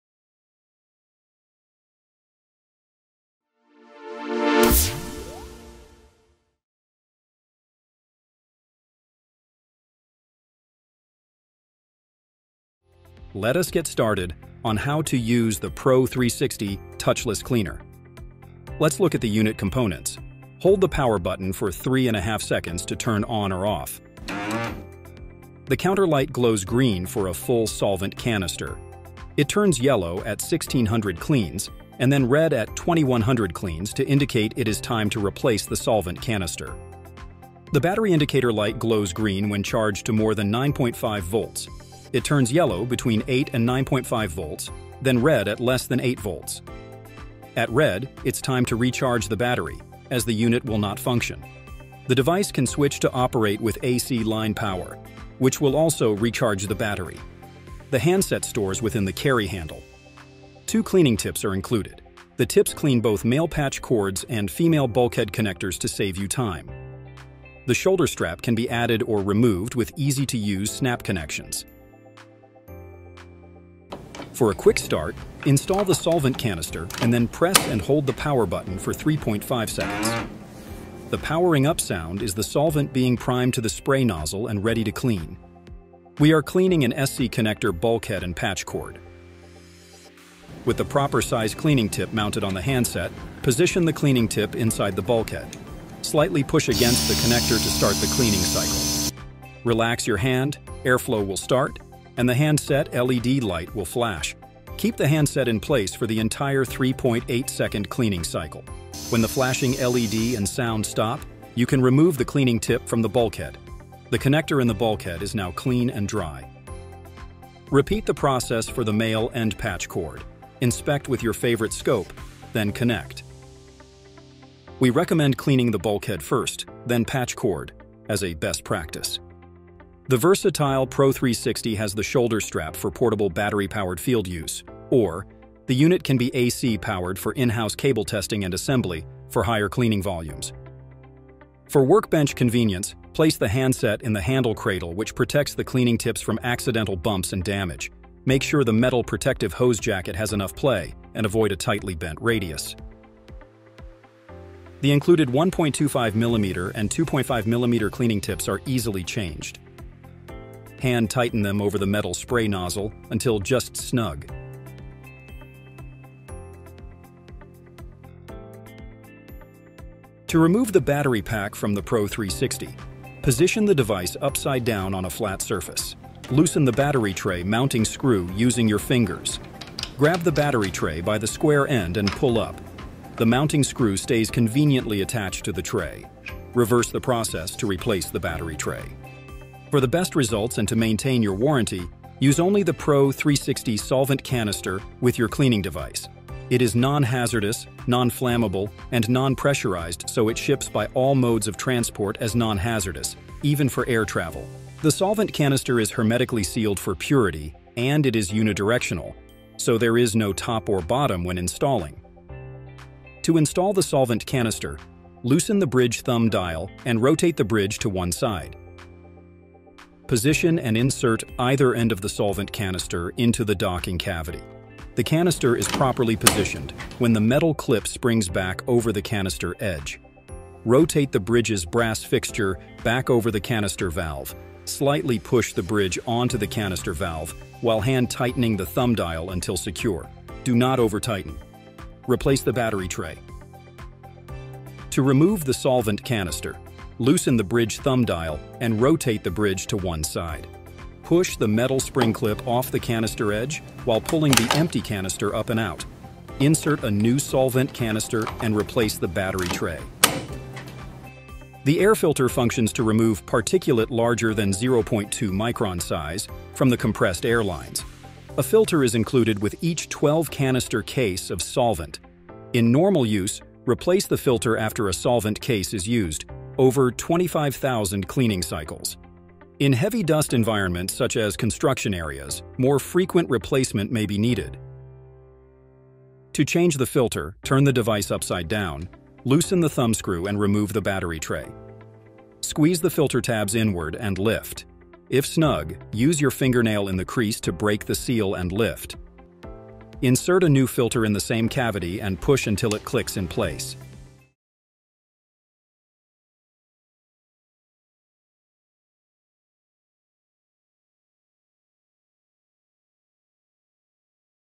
MicroCare Sticklers™ Pro 360°™ Fiber Optic Connector Cleaner - Available from Fiber Optic Center
The MicroCare Sticklers™ Pro360°™ Connector Cleaner is an advanced fiber optic end face cleaner designed for high performance. Simply pressing the connector cleaner handset, efficiently removes oils and dust from the entire end face of both male and female connector ends. This portable fiber cleaning device is a first in the fiber optic industry, offering best-in-class benefits by spraying a high-purity solvent onto the connector end face for non-contact cleaning.

Intro - 0:00
Components - 0:20
Quick Start Demo - 1:11
Removing Battery Pack - 2:48
Fluid Canister - 3:09
How to Install Canister - 3:32
Removing Canister - 4:03
Air Filter - 4:21
Change Air Filter - 4:45
Outro - 5:04